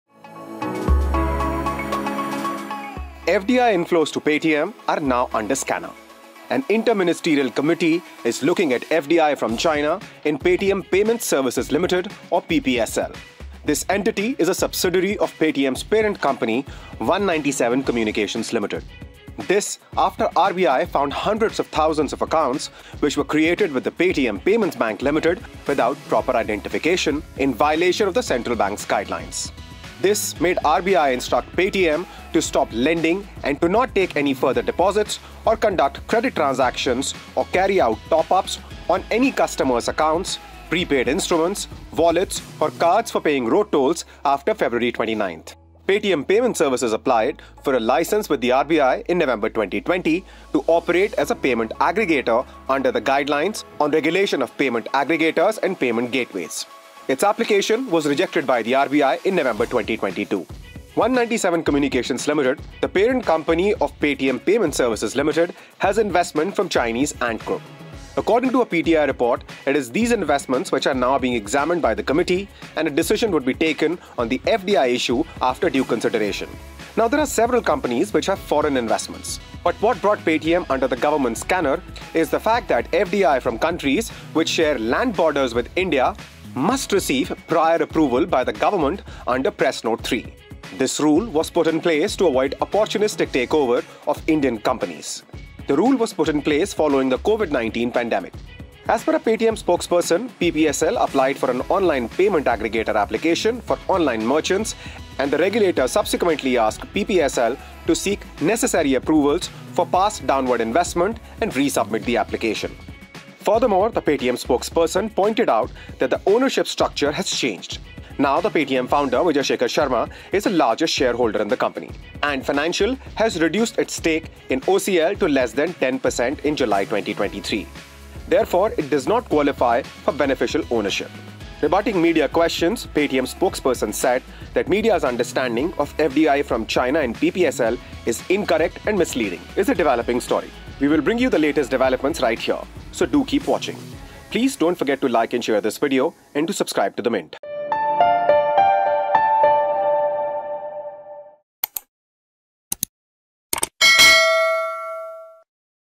Government Probing Chinese FDI In Paytm Payments Services | Report
FDI inflows to Paytm are now under scanner. An inter-ministerial committee is looking at FDI from China in Paytm Payments Services Limited or PPSL. PPSL is a subsidiary of Paytm’s parent company One97 Communications Limited.

This after RBI found hundreds of thousands of accounts which were created with Paytm Payments Bank Limited without proper identification, in violation of the central bank’s guidelines.